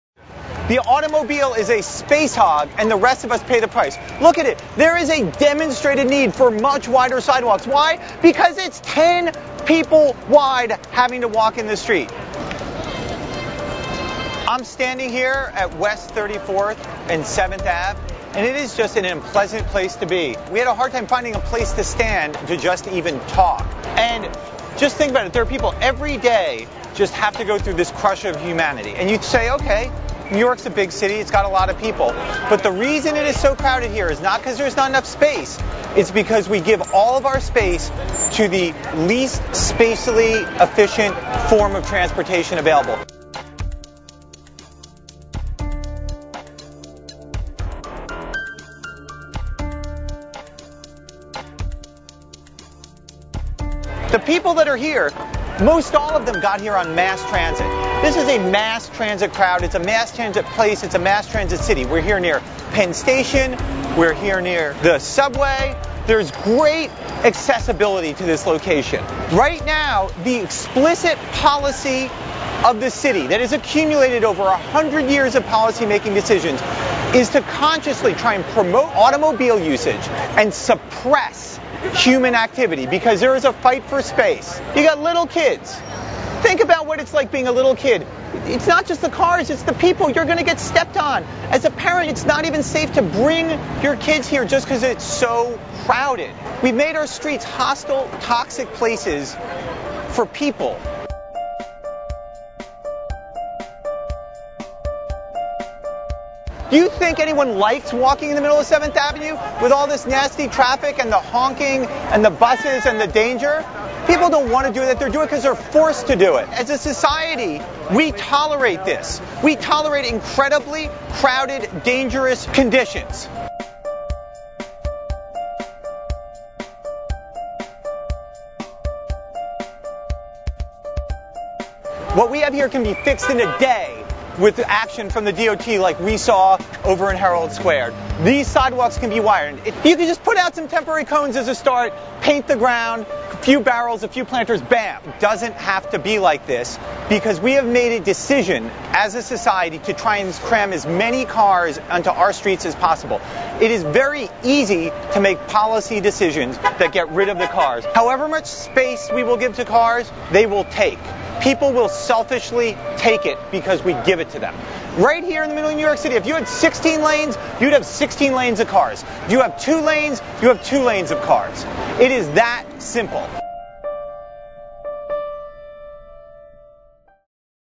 Streetfilms-Pedestrian Overcrowding: 34th St/7th Avenue
Mark Gorton takes a visit to a corner of Herald Square that becomes choked with people during rush hours.  He posits that we can better use our space if we take away space devoted for cars and re=purposed it for people.